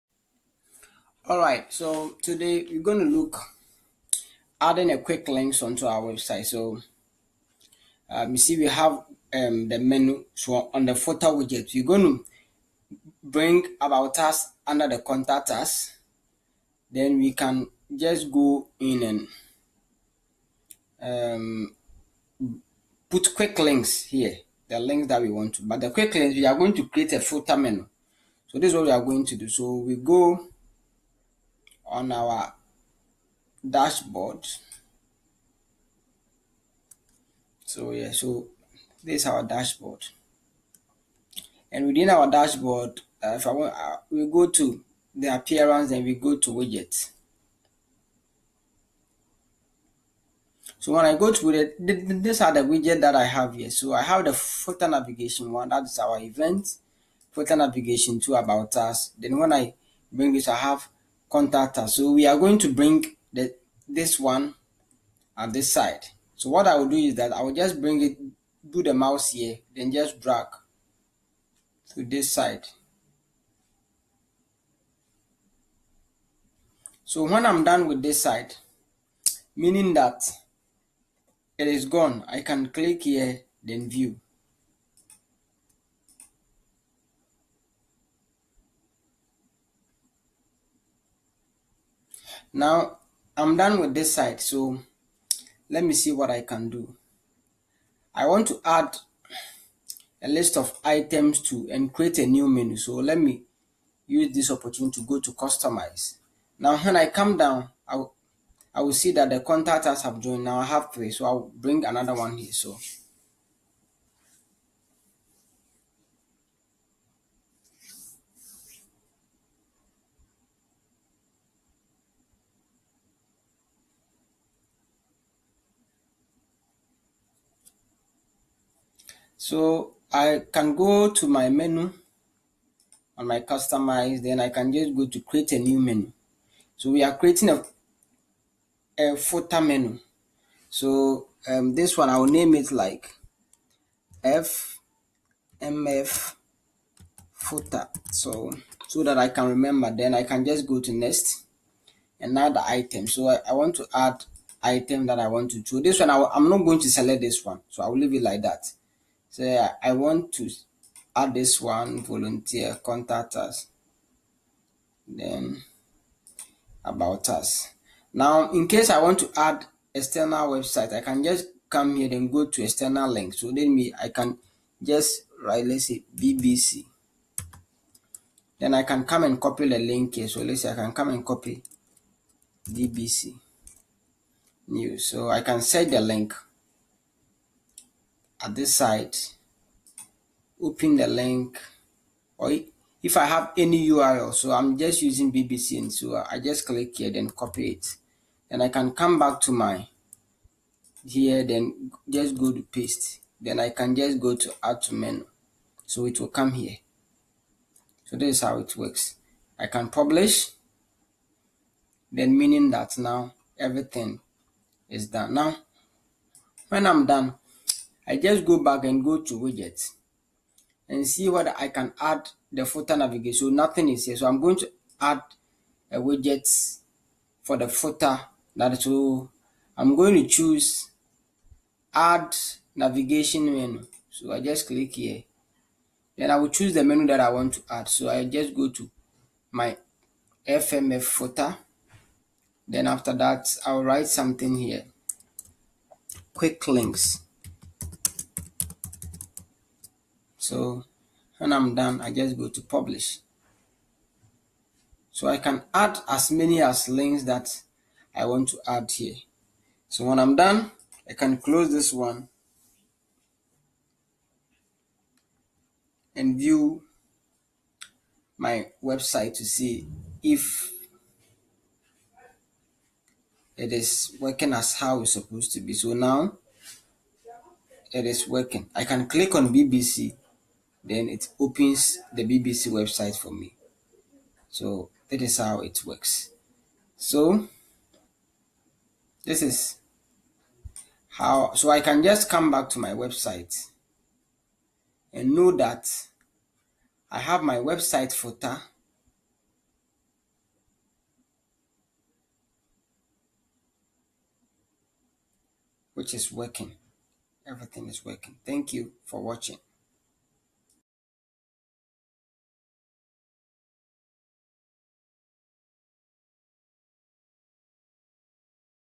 How to create Footer QUICK LINKS and Footer Menu
This Lesson will help you to create Quick Links and Footer Menu.
Continue and enjoy more lessons.